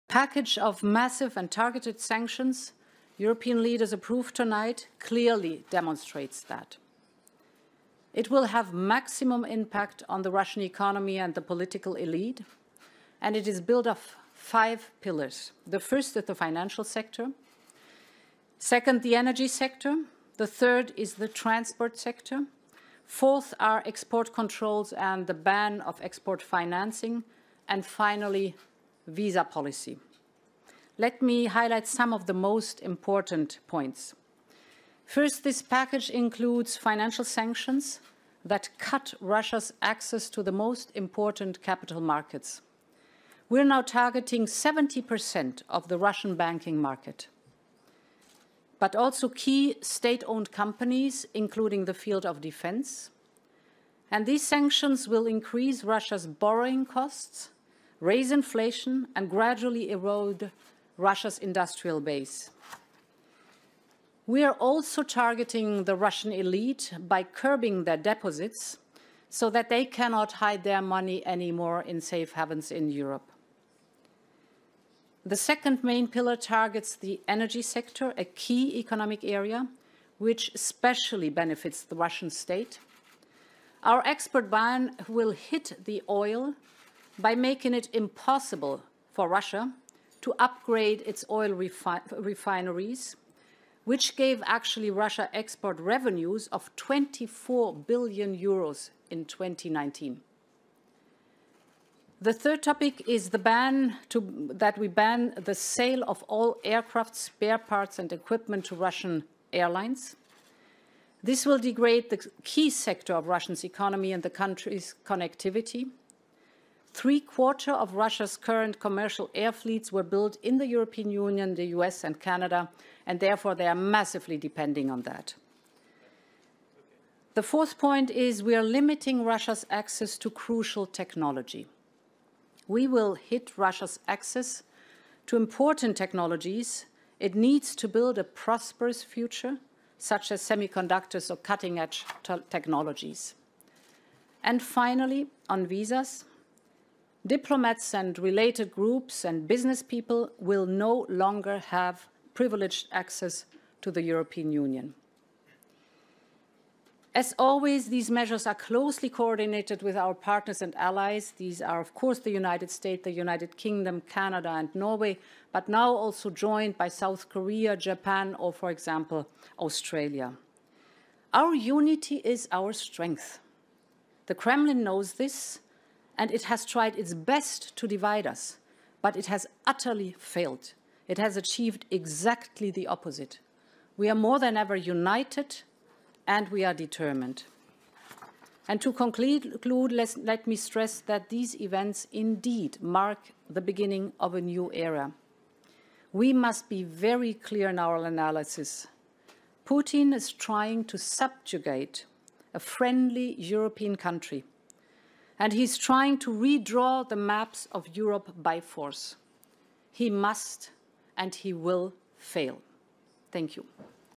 Ursula von der Leyen po zasadnutí Európskej rady 24. februára 2022.
Včera v noci schválili európski lídri balík rozsiahlych a cielených sankcií, ktorých cieľom je tvrdo zasiahne ruské hospodárstvo a jeho politickú elitu.

Sankcie pozostávajú z piatich pilierov:

1. finančné sankcie, ktoré obmedzujú prístup Ruska na najdôležitejšie kapitálové trhy. Zameriavame sa na 70 % ruského bankového trhu, ale aj na kľúčové štátom vlastnené spoločnosti vrátane oblasti obrany. Tieto sankcie zvýšia ruské náklady na prijaté úvery a pôžičky, zvýšia infláciu a postupne narušia priemyselnú základňu Ruska. Sankcie sú zamerané aj na ruskú elitu – obmedzujú jej vklady, aby už nemohla skrývať svoje peniaze v Európe.

2. Sankcie sú zamerané tiež na odvetvie energetiky - kľúčovú hospodársku oblasť, z ktorej má prospech najmä ruský štát. Zákaz vývozu zasiahne ropu, Rusko tak nebude môcť modernizovať svoje ropné rafinérie. Ruské príjmy z vývozu ropy v roku 2019 predstavovali 24 miliárd EUR.

3. Zákaz predaja všetkých lietadiel, náhradných dielov a vybavenia pre ruské letecké spoločnosti.

4. Obmedzujeme prístup Ruska k kľúčovým technológiám, ako sú polovodiče alebo špičkové technológie.

5. Víza - diplomati a súvisiace skupiny, ako aj podnikatelia už nebudú mať privilegovaný prístup do Európskej únie.

Pozrite si časť prejavu predsedníčky Európskej komisie Ursuly von der Leyenovej na spoločnej tlačovej konferencii s predsedom Európskej Rady Michelom a prezidentom Macronom po mimoriadnom zasadnutí Európskej rady 24. februára 2022.